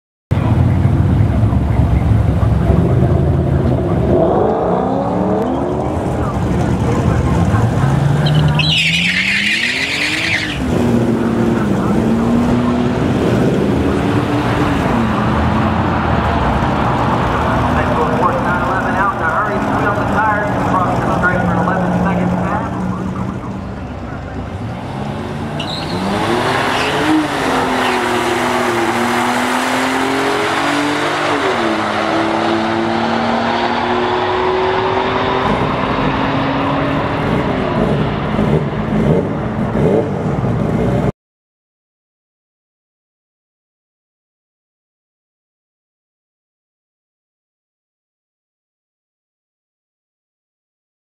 911 Turbo (996) runs 11.8 @ 124 in a 1/4 Mile Drag Race - Tiptronic
A tuned Tiptronic 911 Turbo Porsche runs a respectable high 11 second quarter mile pass at 124mph. Check out that four wheel spin at the launch.